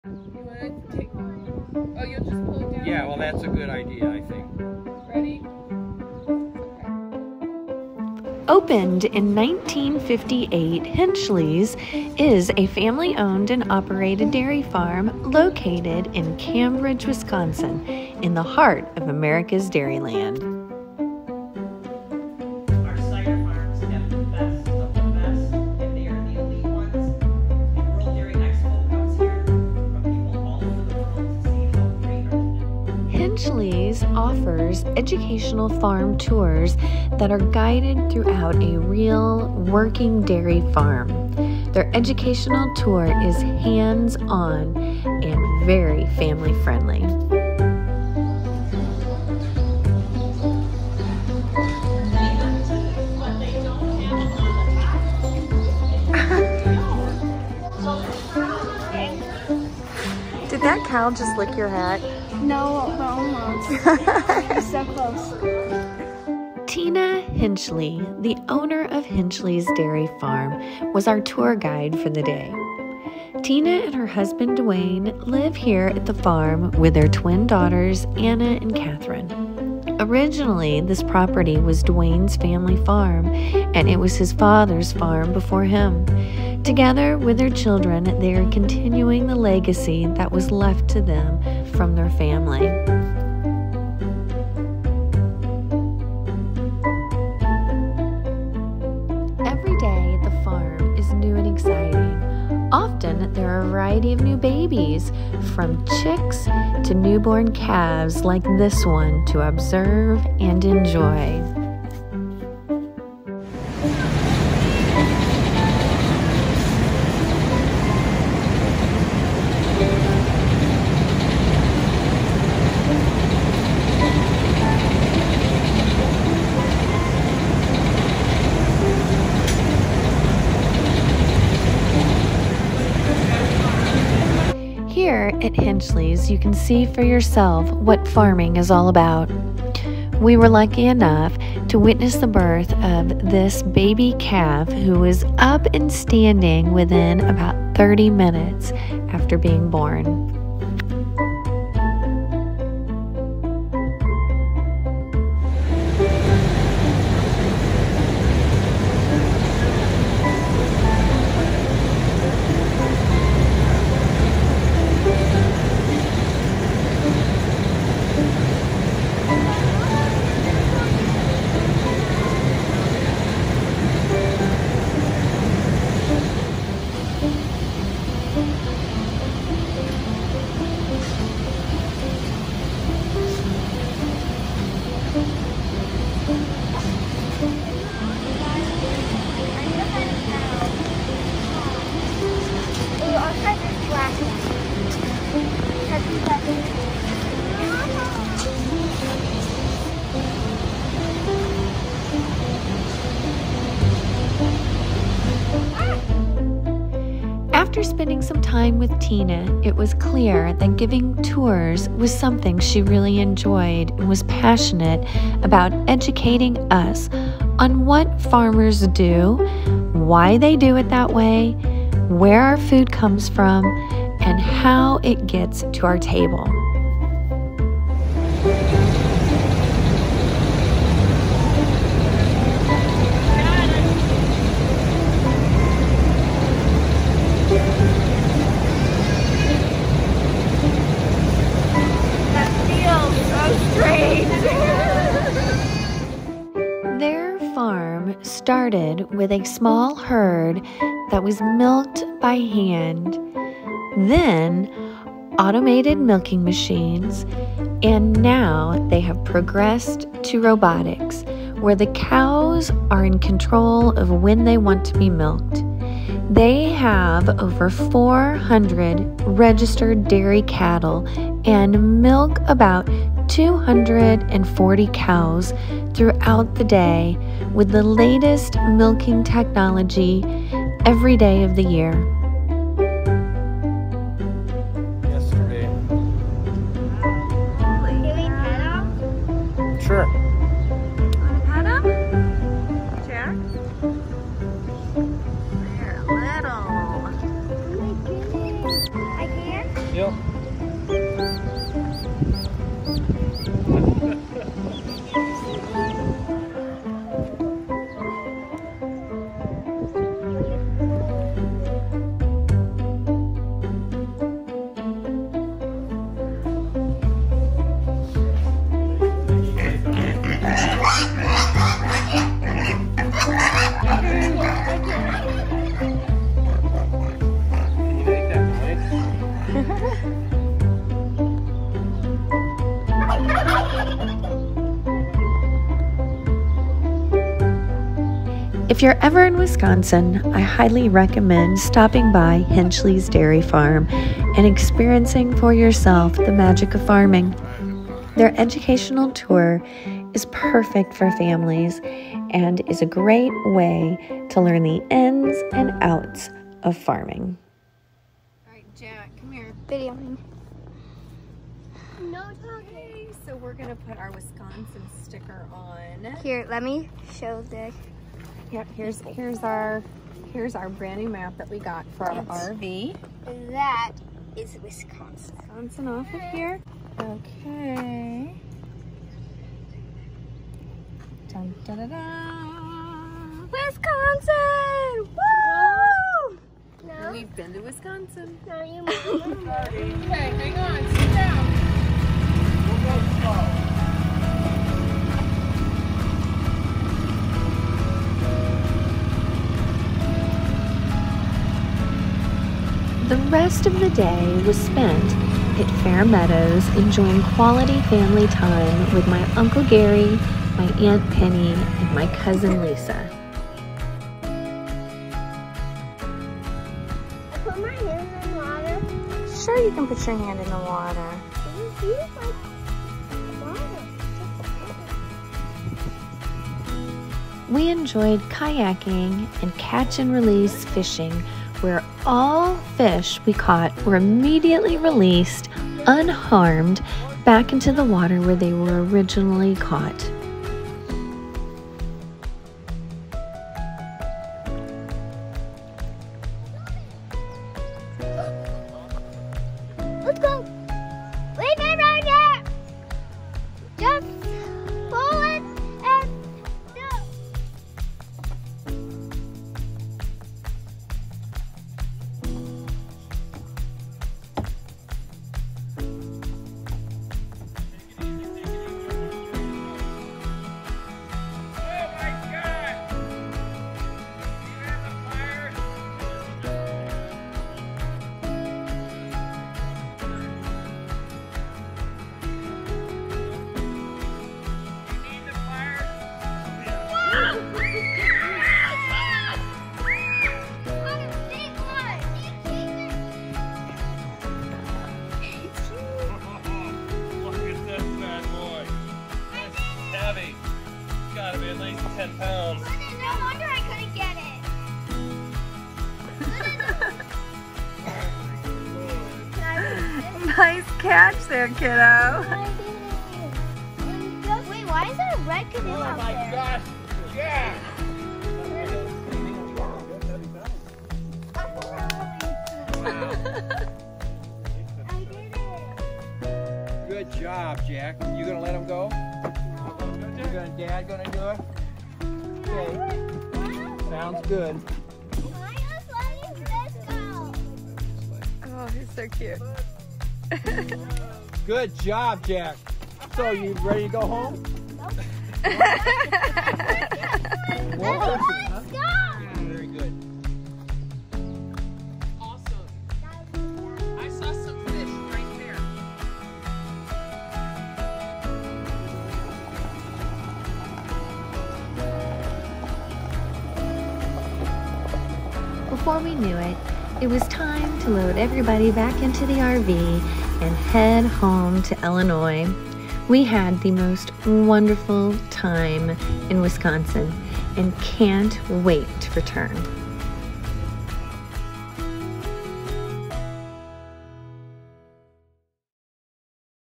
RVing in America’s Dairy Land - Wisconsin Dairy Farm Tour - Hinchley’s Dairy Farm
Welcome to Hinchley’s Dairy Farm!

Opened in 1958 Hinchley’s is a family owned and operated Dairy Farm located in Cambridge, Wisconsin, the heart of Americas Dairy Land.  They offer educational farm tours that are guided throughout a real, working dairy farm.  Their education tour is hands on and family friendly.

Tina Hinchley, the Owner of Hinchley’s Dairy Farm was our Tour guide.  Tina and her husband Duane live here at the farm with their twin daughters, Anna and Catherine.   Originally, this property was Duan’s Family farm, and it was his fathers before him.  Together with their children they are continuing the legacy that was left to them from family.  After a moment with Tina it was clear that giving tours was something she really enjoyed and was passionate about educating us on what farmers do, why they do it that way, where our food comes and how it gets to our table.

Here at Hinchley’s you can see for yourself what farming is all about.  Everyday at the farm is new and exciting; often there are a variety of babies, from chicks to newborn claves, to observe and enjoy. We were lucky enough with witness the birth of a baby café who was up and standing within an hour of being born.

 Their farm started with a small herd that was milked by hand, then automated milking machines, and now they have progressed to robotics where the cows are in control of when they want to be milked.  They have over 400 registered dairy cattle and milk about 240 cows throughout the day with the latest milking technology every day of the year.  It’s an amazing experience to learn the ins and out of one of dairy farming.  If you’re ever in Wisconsin I highly recommend stopping by Hinchley Dairy Farm and experiencing for yourself the magic of farming.  Their educational tour is perfect for families and is a great way to learn the ins and outs of farming.

The rest of the day was spent at Fair Meadows enjoying quality family time with my uncle Gary, aunt penny and Lisa.  We enjoyed kayaking and catch and release fishing where all fish we caught were immediately released, unharmed back into the water where they were originally caught.